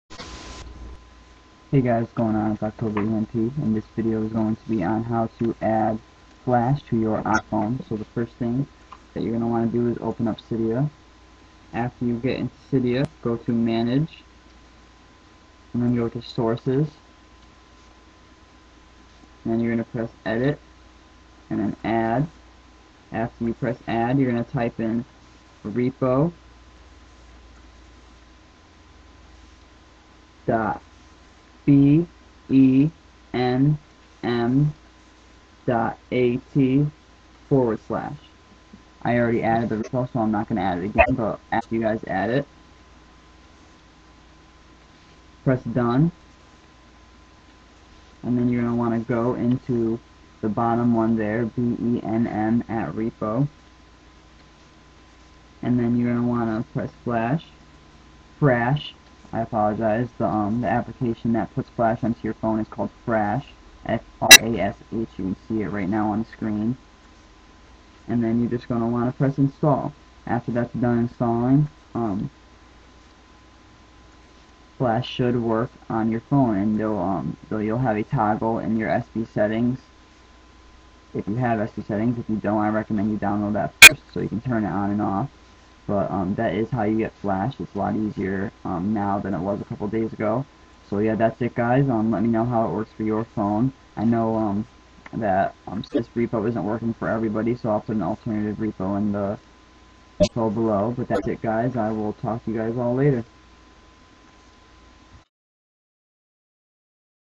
How to: Get Flash (Frash) on you iPhone (NO SSH)
Hey here is how to get the cydia application frash which imports flash onto your iPhone, i hear it is working pretty well. though it doesnt work with all the flash content on the internet it does pretty well for being a 3rd party tweak.

the other repo was removed from the website so my guess it was removed...let me know how it works for you guys